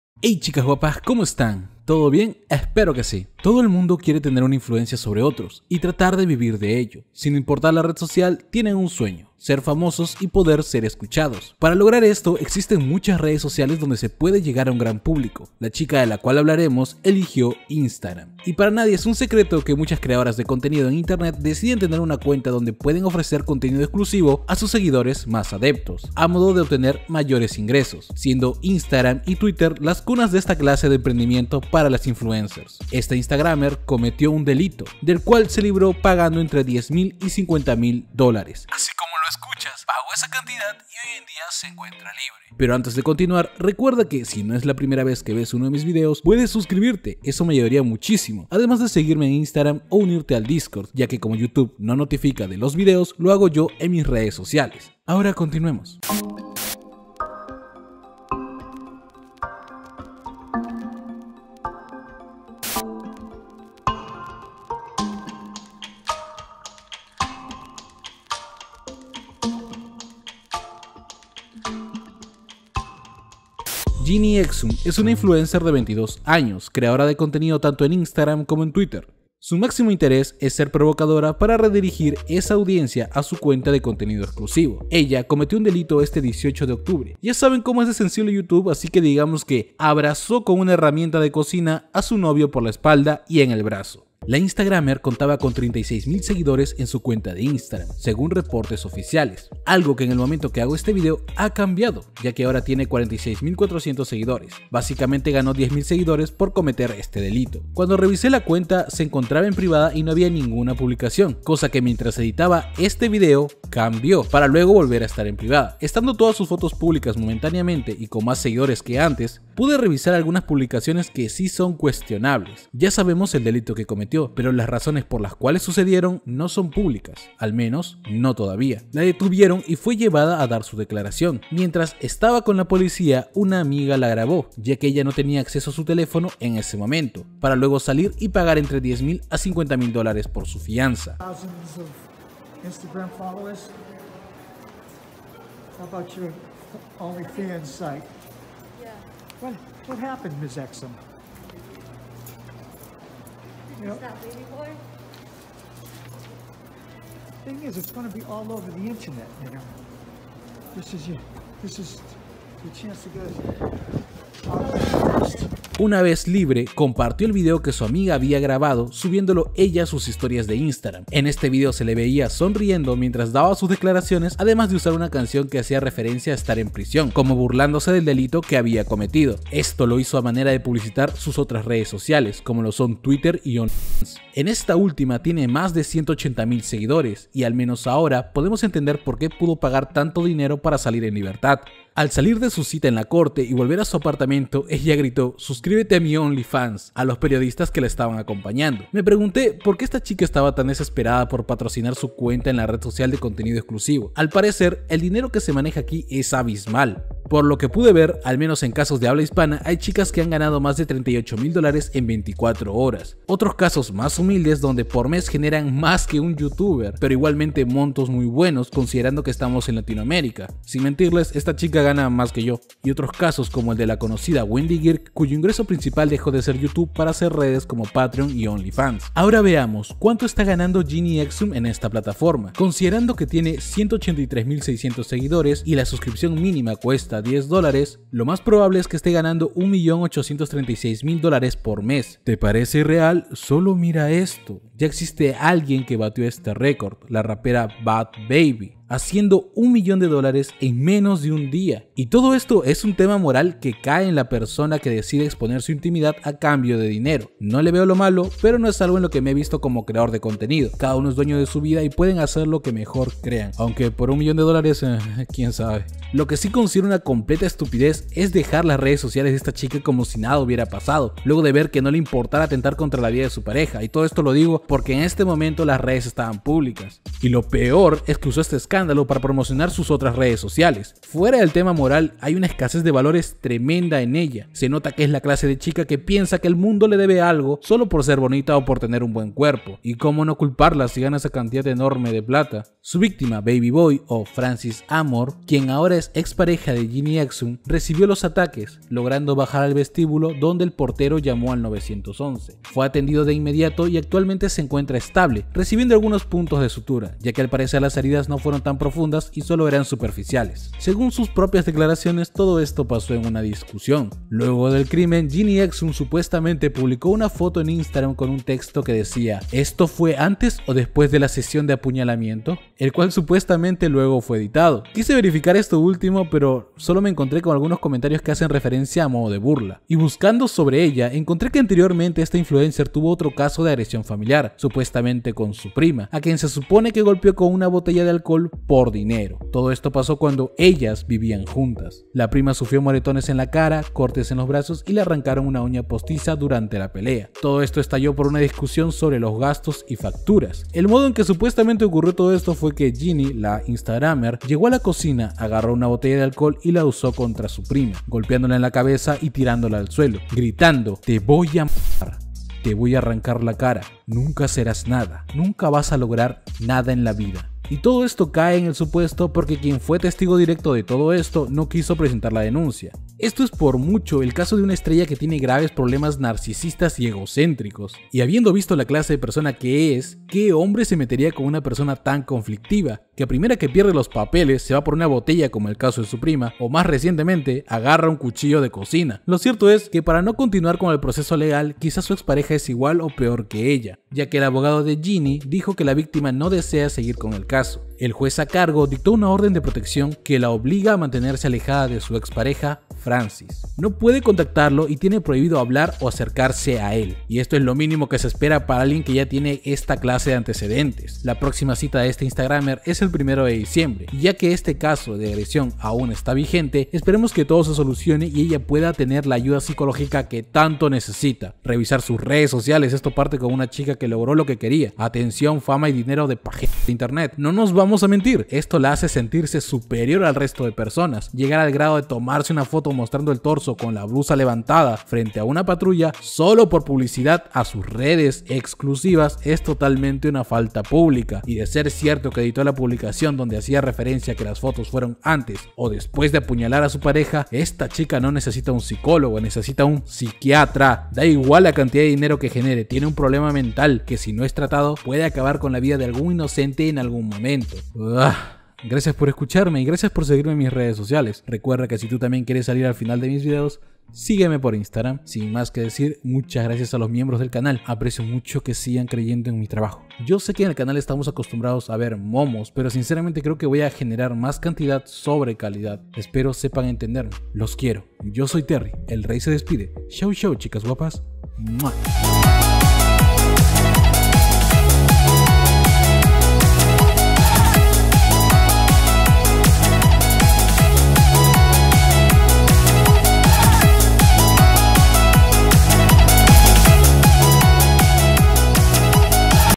La INFLUENCER que INTENTÓ ACABAR con su NOVIO por VISITAS | Genie Exum
Hoy hablaremos de Genie Exum quien fue envuelta en una polémica, la conocida modelo de Instagram y redes como Only, cometió un delito en contra de su ex pareja.

Un caso muy turbio sobre las nuevas celebridades que se estan creando
Gini exum era tiktoker e instagramer.